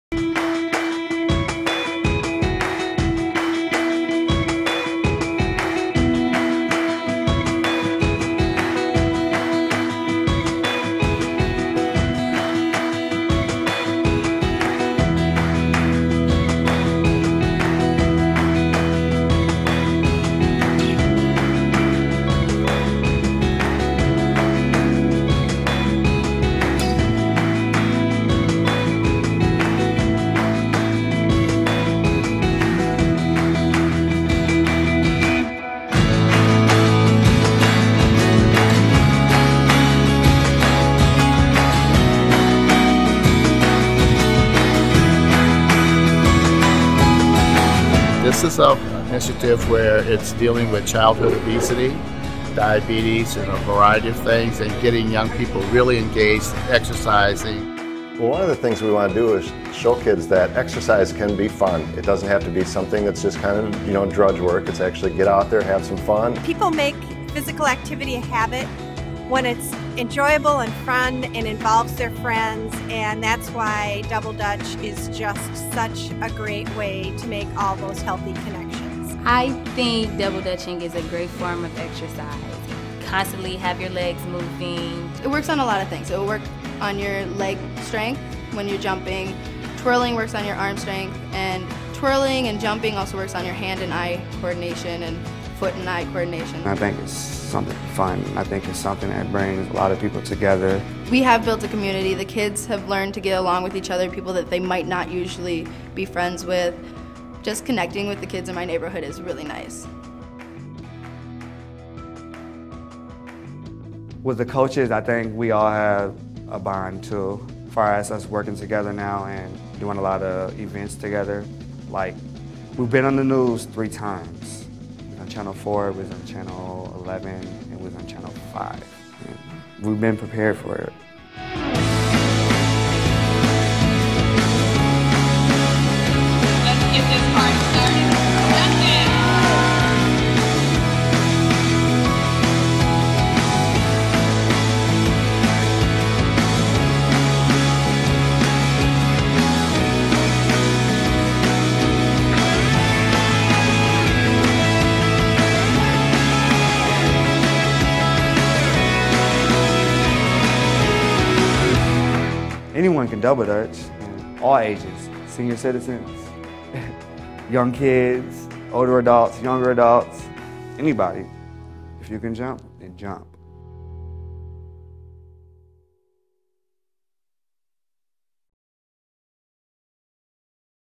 Jump Jam // The Movement
To promote physical activity in our community, HealthPartners partnered with the City of Saint Paul and the Saint Paul YWCA to organize Jump Jam -- a free Double Dutch competition for all ages. The Jump Jam event was part of a nine-week program.